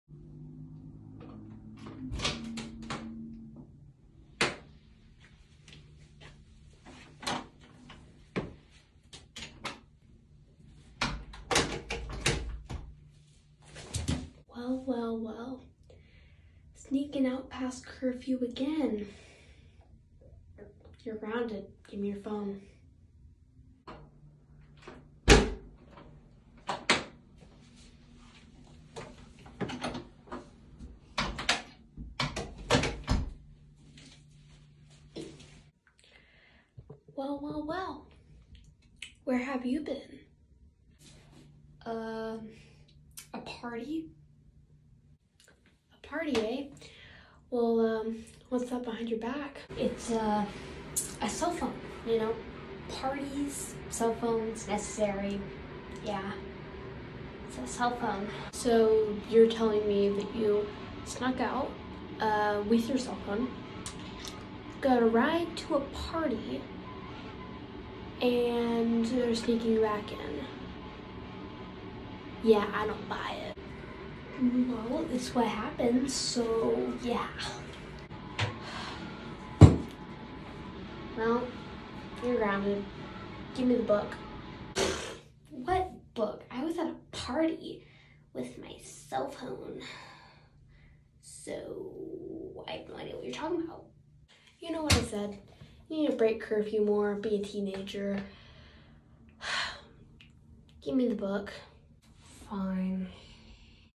My brother sneaking in past curfew vs me
The first one is my brother, the second is me!